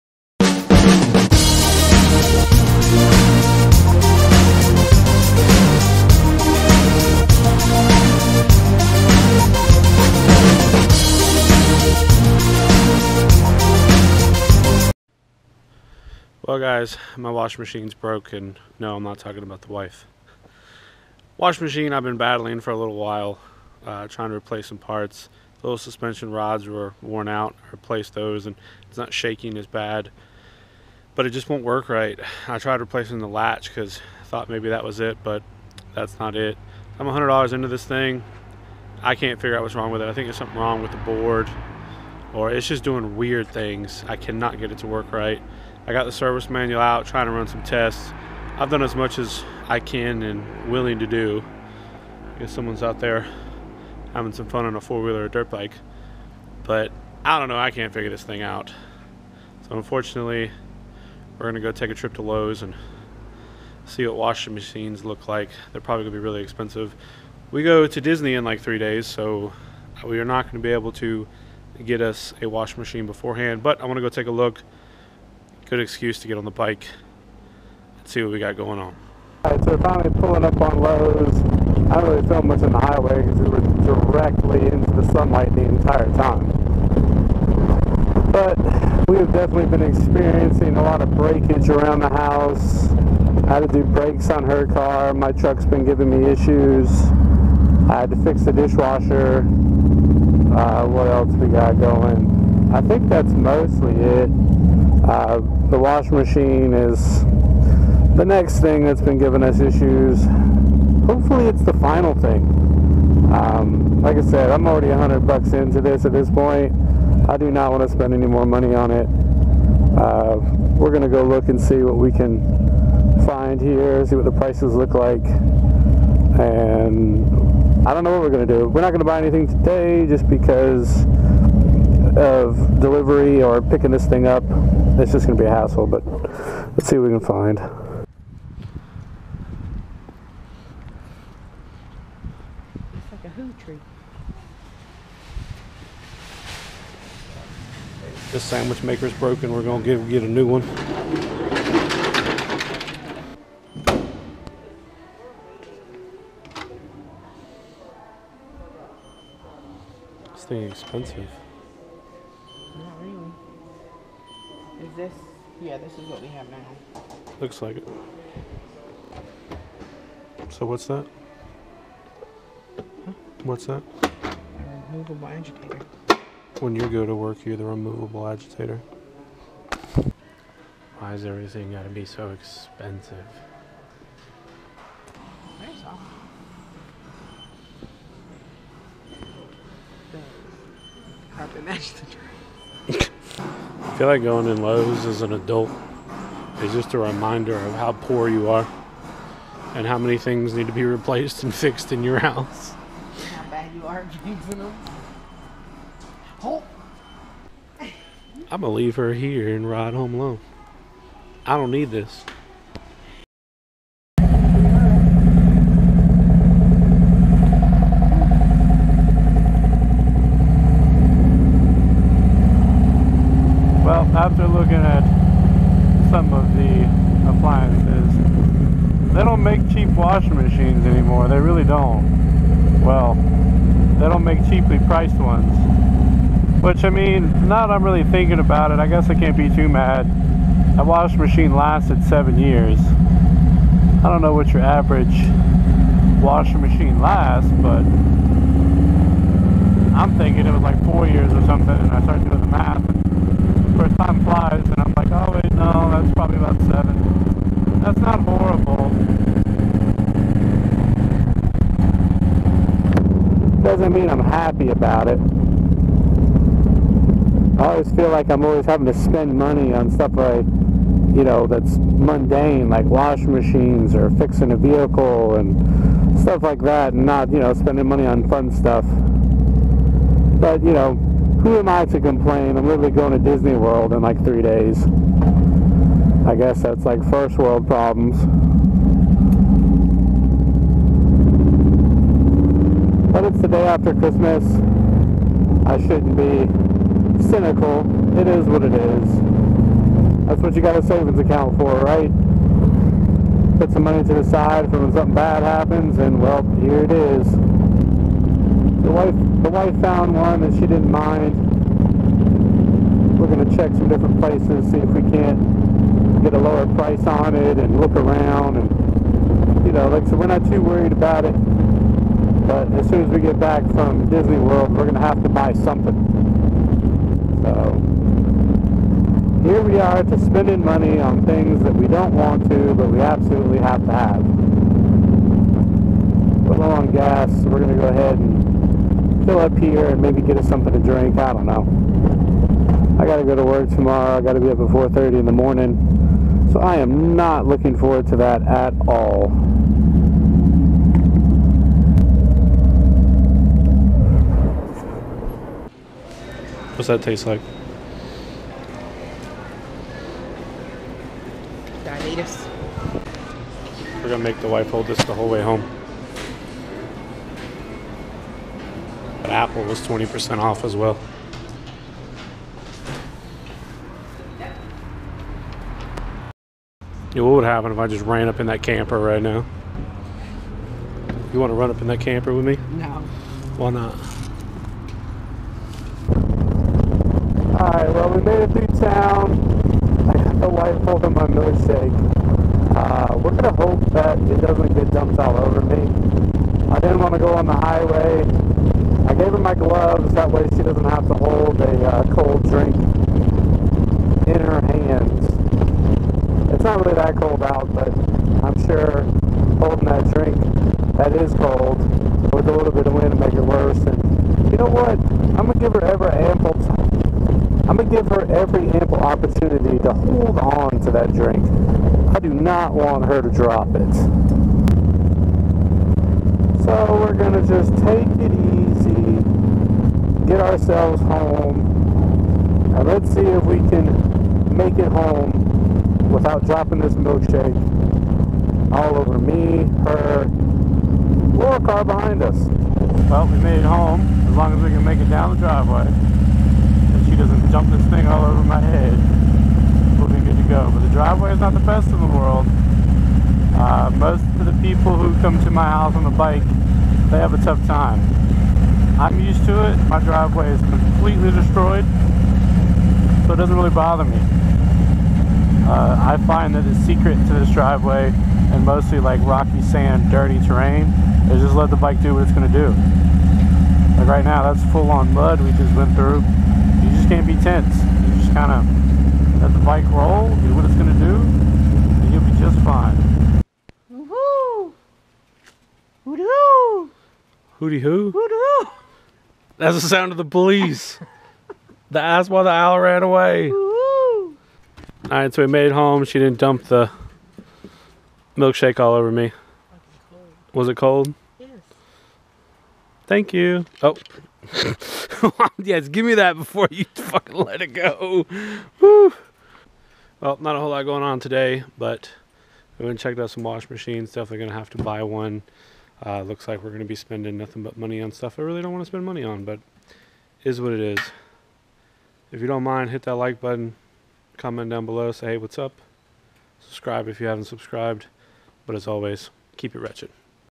Gonna leave my wife!
Well everything is breaking around me so lets head to lowes and see how much a new washer will cost.